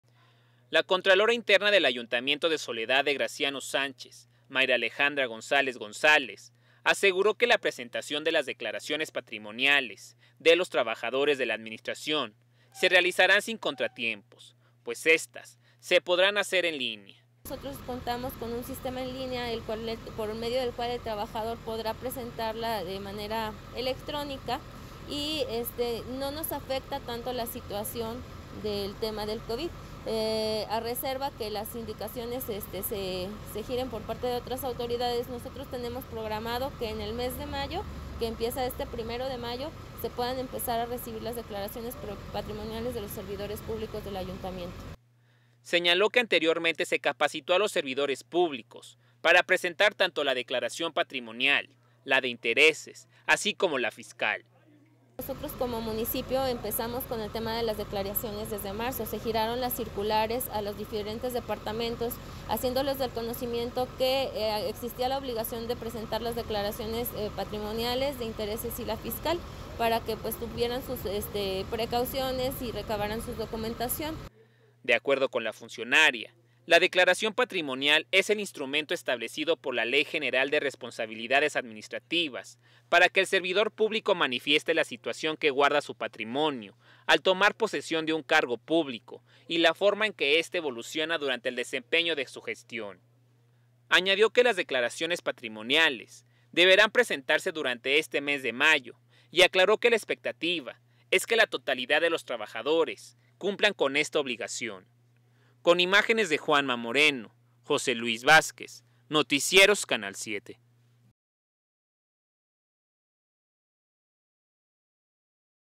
Declaraciones patrimoniales se presentarán sin contratiempos en SGS.
SGS.- La contralora interna del Ayuntamiento de Soledad de Graciano Sánchez, Mayra Alejandra González González, aseguró que la presentación de las declaraciones patrimoniales de los trabajadores de la administración se realizarán sin contratiempos, pues éstas se podrán que hacer en línea.

"Nosotros contamos con un sistema en línea por medio del cual el trabajador podrá presentarla de manera electrónica y no nos afecta tanto la situación del tema del Covid-19, esto a reserva de las indicaciones que se giren por parte de otras autoridades; tenemos programado que en el mes de mayo se pueda empezar a recibir las declaraciones patrimoniales de los servidores públicos del ayuntamiento", dijo.

Señaló que anteriormente se capacitó a los servidores públicos para presentar tanto la declaración patrimonial, la de intereses, así como la fiscal.

"Como municipio empezamos con el tema de las declaraciones desde marzo, se giraron las circulares a los diferentes departamentos haciendo de ese conocimiento que existía la obligación de presentar las declaraciones patrimoniales de intereses y la fiscal para que tuvieran sus precauciones y recabarán su documentación", agregó.

De acuerdo con la funcionaria, la declaración patrimonial es el instrumento establecido por la ley general de responsabilidades administrativas para que el servidor público manifieste la situación que guarda su patrimonio al tomar posesión de un cargo público y la forma en que esté evoluciona durante el desempeño de su gestión.

Añadió que las declaraciones patrimoniales deberán presentarse durante este mes de mayo e indicó que la expectativa es que la totalidad de los trabajadores cumplan con esta obligación.